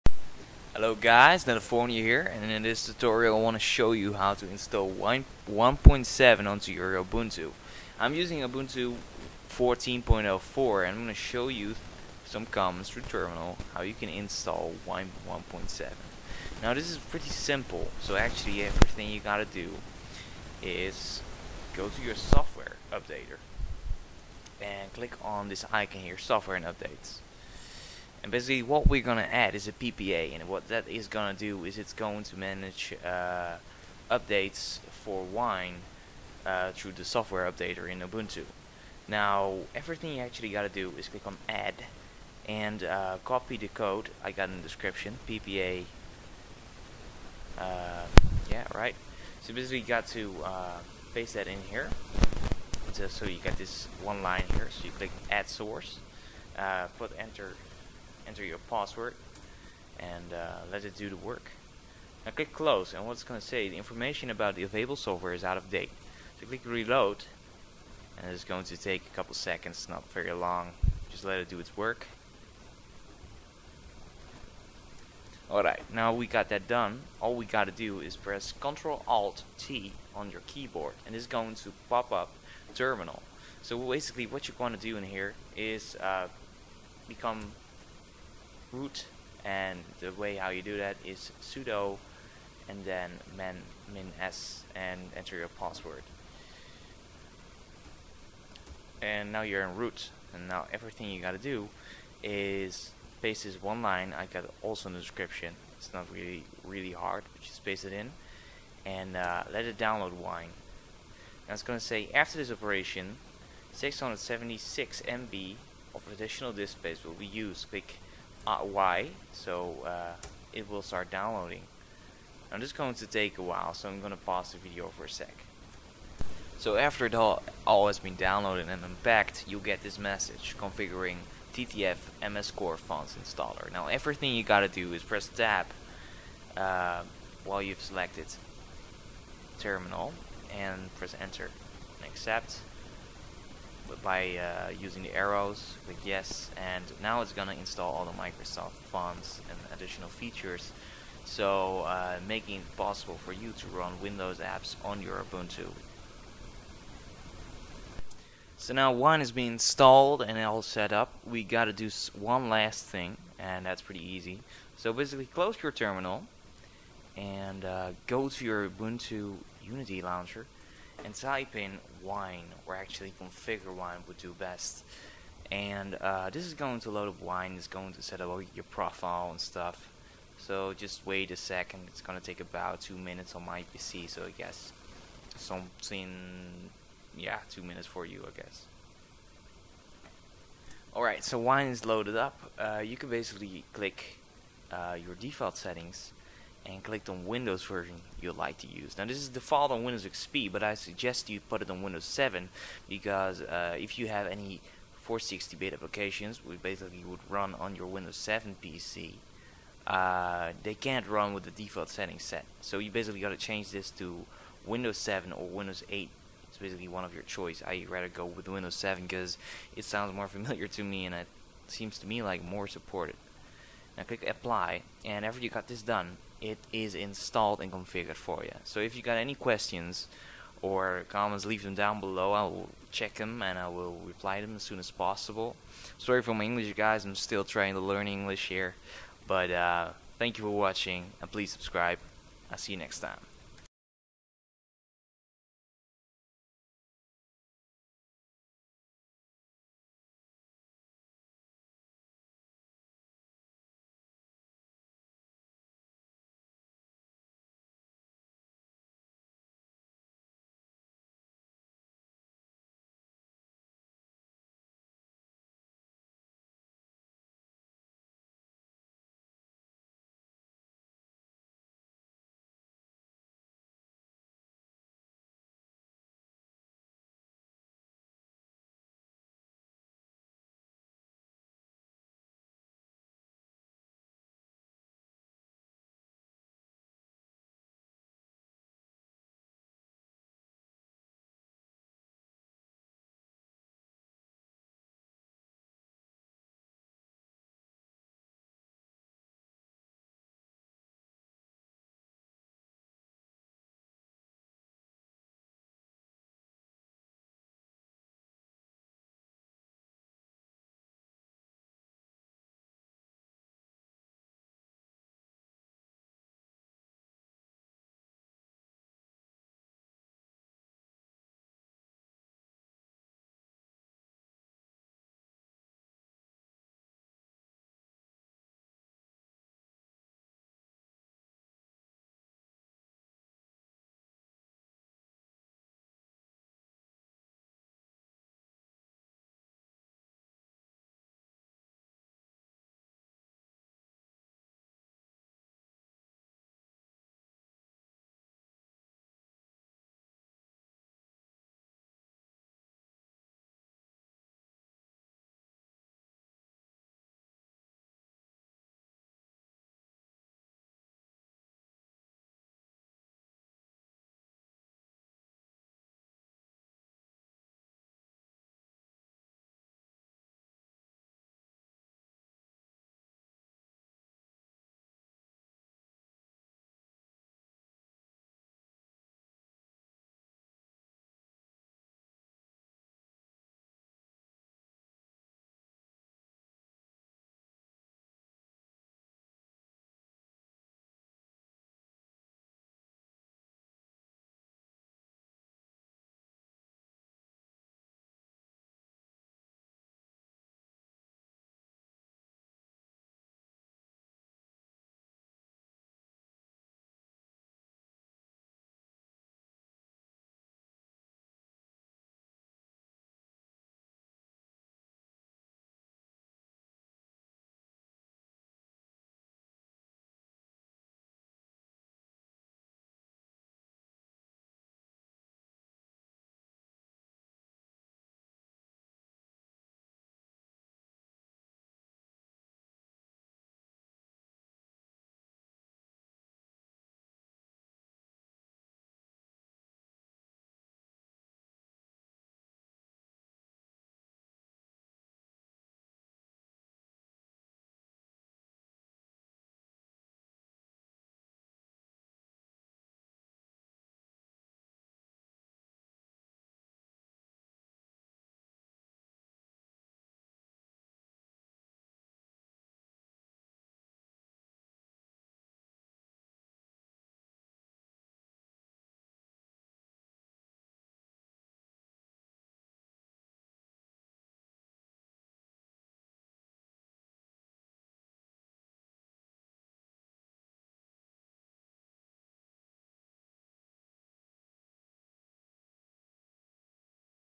How to install Wine 1.7 on Ubuntu
Copy & paste:
ppa:ubuntu-wine/ppa
sudo apt-get install wine1.7